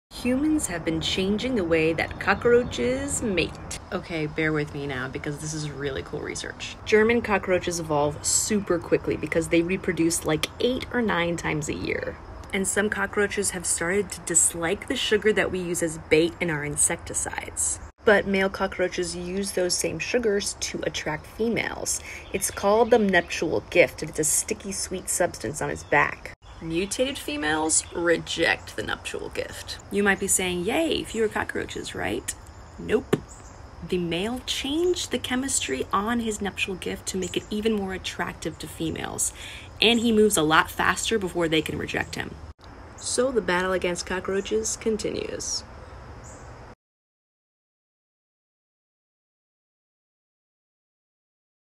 The change in cockroach reproduction 🪳🫣
German cockroaches evolve quickly. Here’s how our baits have affected their sex lives.

Watch the full story on 10/4:  @SciNC